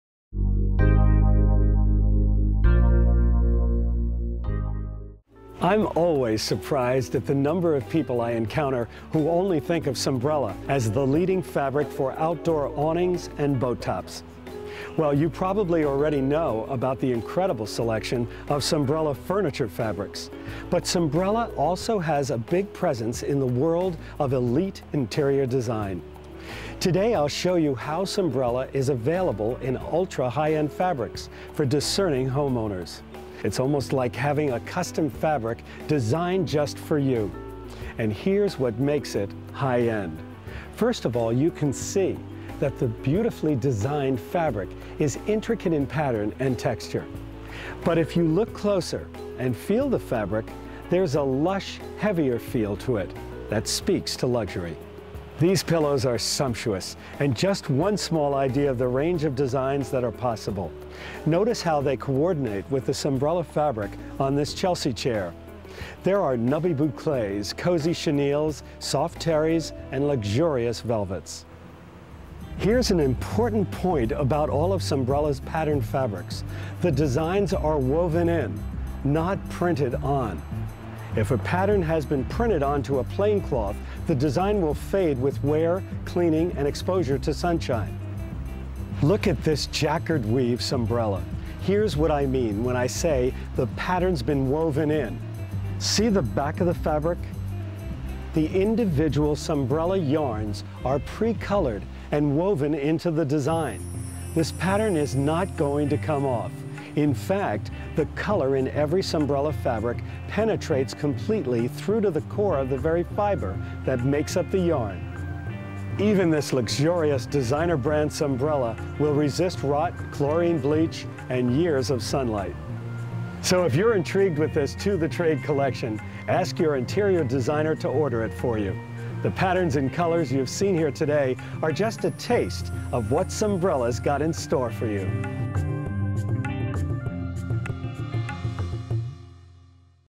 Beautiful Sunbrella Fabrics
Joe Ruggiero, designer HGTV host discusses the wide variety of Sunbrella fabrics.